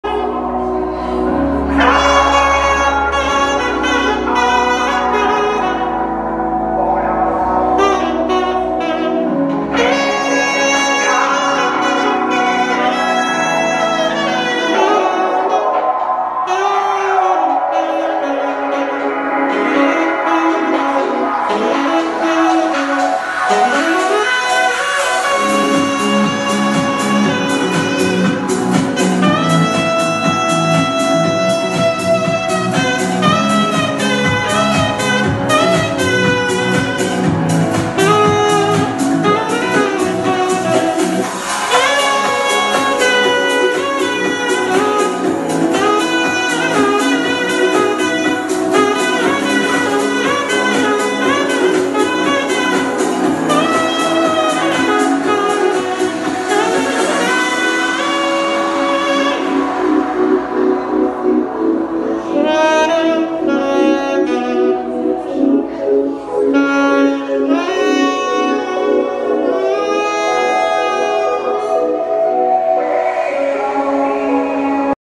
Boquilha Taigor Classic 7 Sax Tenor - Davi Calixto Sax
Uma Boquilha com som quente, projetada com uma pequena câmara, com defletor elevado e perfuração grande para maior impacto sonoro e uma tonalidade cortante, comparável as boquilhas metálicas. Extremamente versátil do Swing ao Jazz. Indicada para músicos iniciantes e avançados.